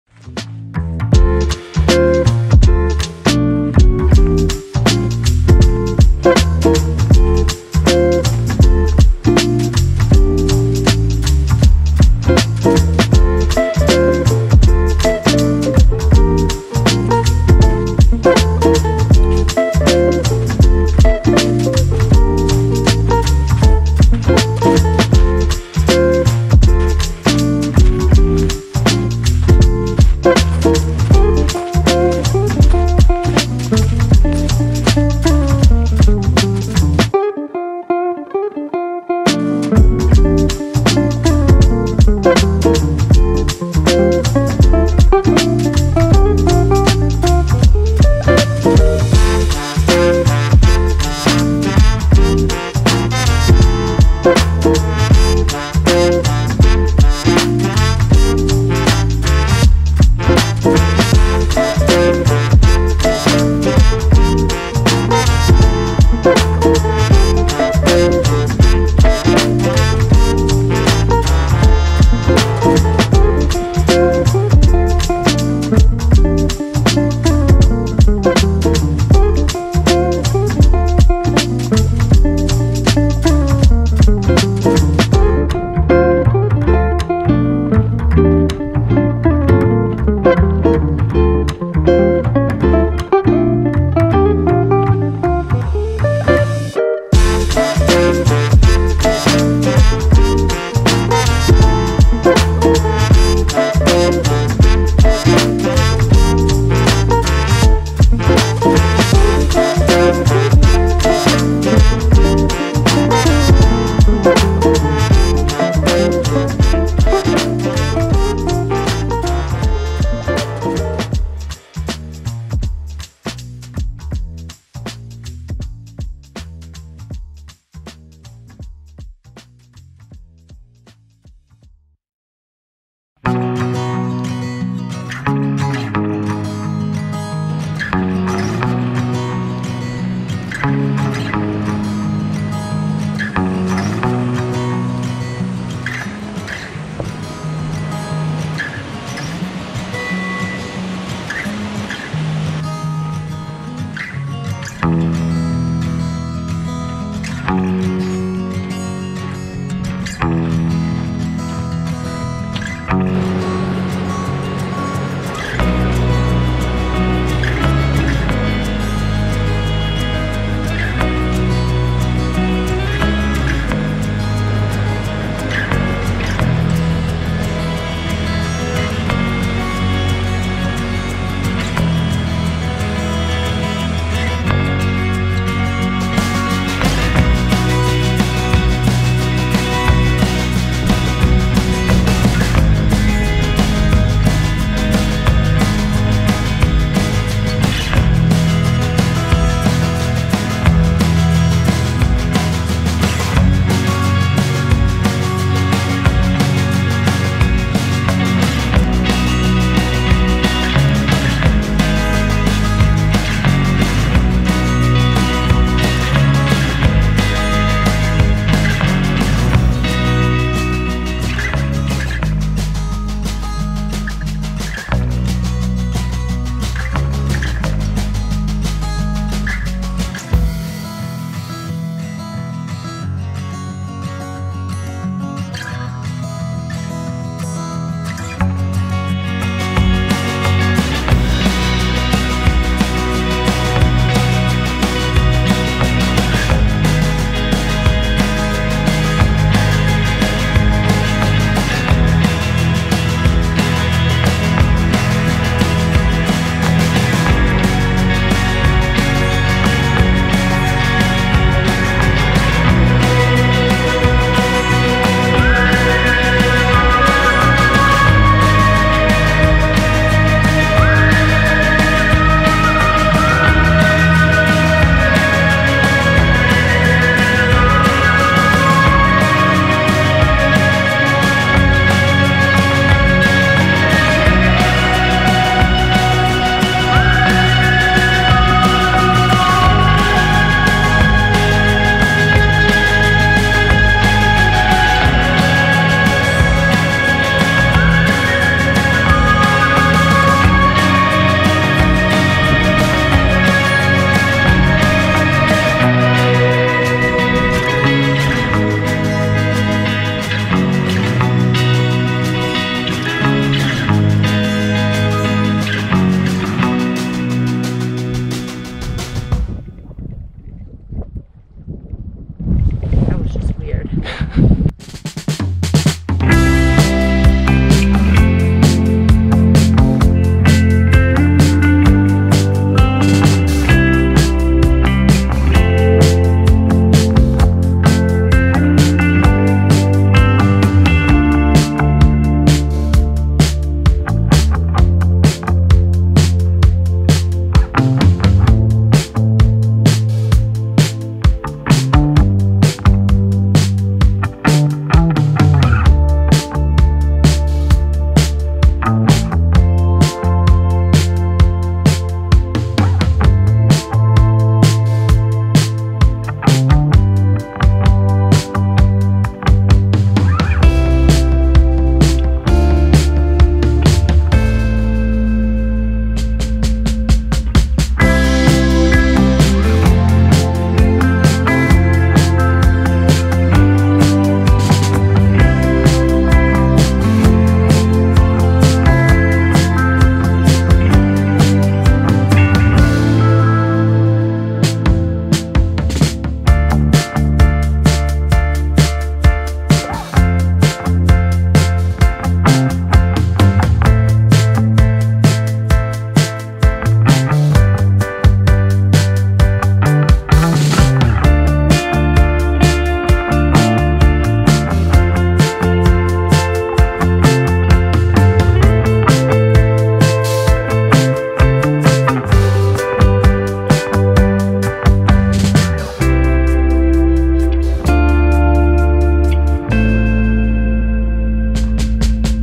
Devils Postpile to Yosemite (JMT Section Hike) + GoPro Hero 7 Battery Fail
Backpacking in 2019 from Devils Postpile National Monument near Mammoth Lakes to Yosemite National Park northbound on the John Muir Trail. GoPro Hero 7 batteries died on day 2! This was my first time using the camera and I made the rookie mistake of shooting in 1920 x 1440 instead of 1920 x 1080. So I had to choose between vertical bars and keeping the aspect ratio or the whole thing looking a bit zoomed in.